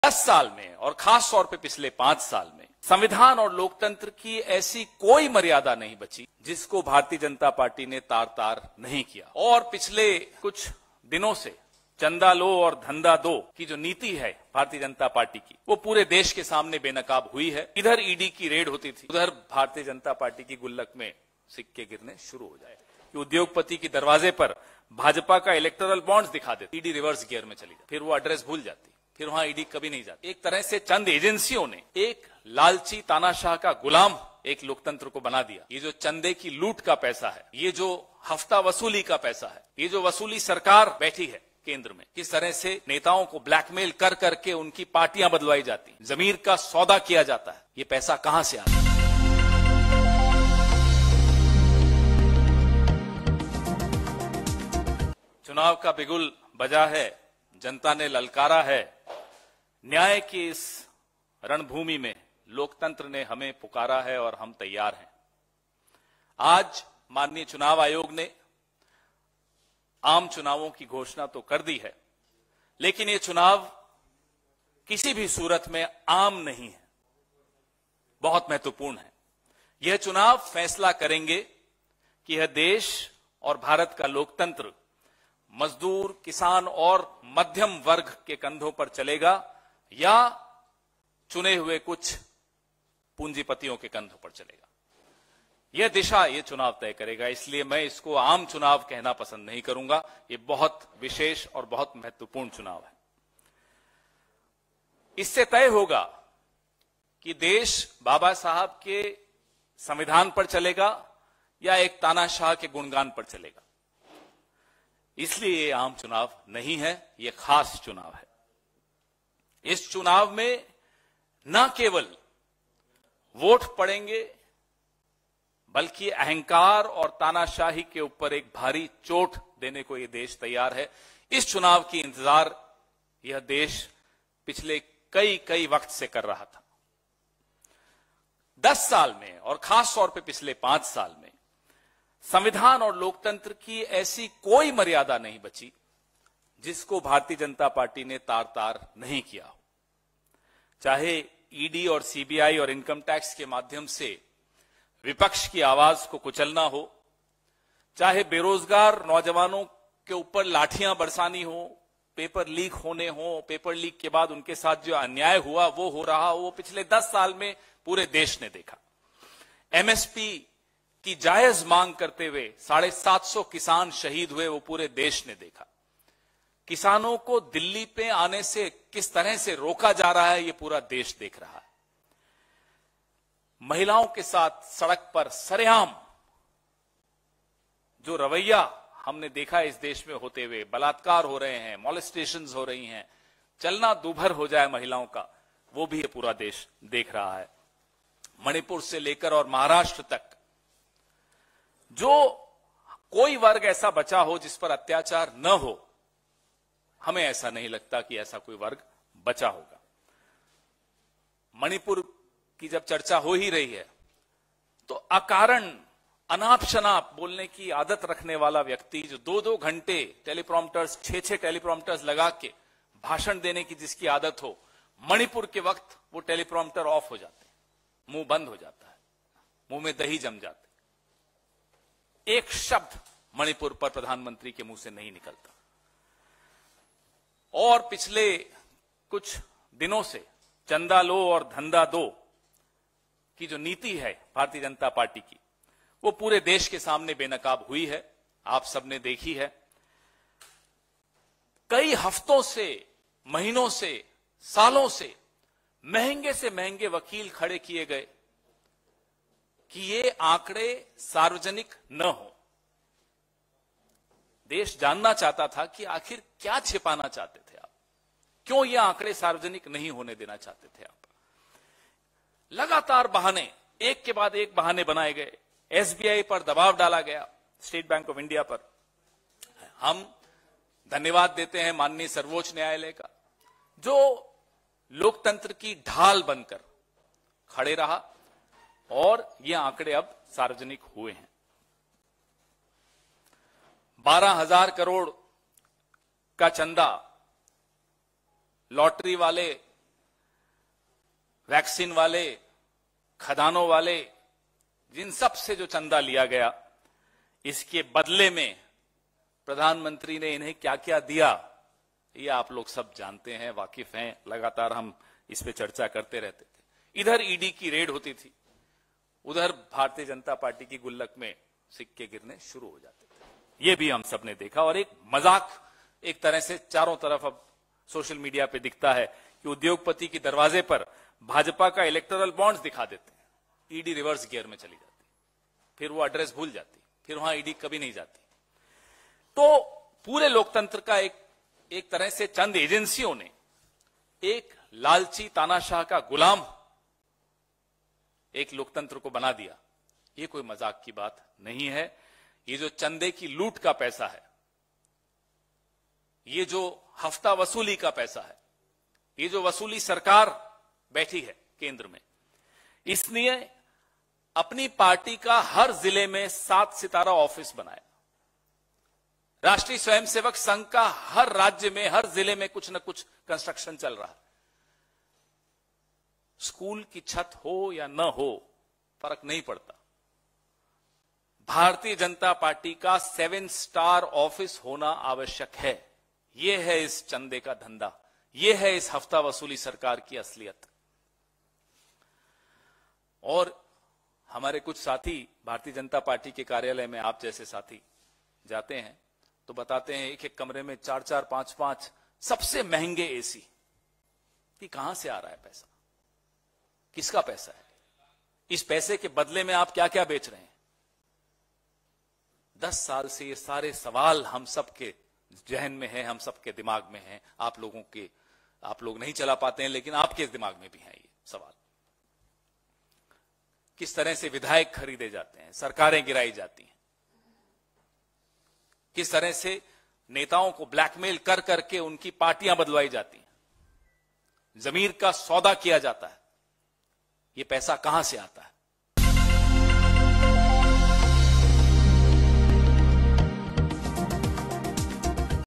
पवन खेड़ा के खुलासे से बीजेपी परेशान /PAWAN KHERA ON ELECTORAL BONDS AND LOK SABHA ELECTION 2024
Video Courtesy -
Produced By - Janhit Awaz
Reporter -